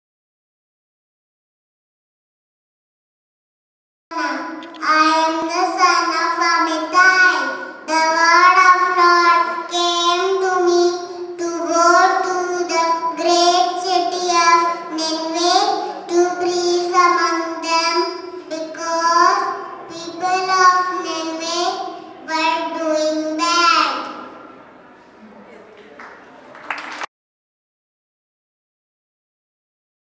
Malayalam Bible Story - Serah Mathew
Malayalam Bible Story - Serah Mathew, St Peters Cathedral, New Delhi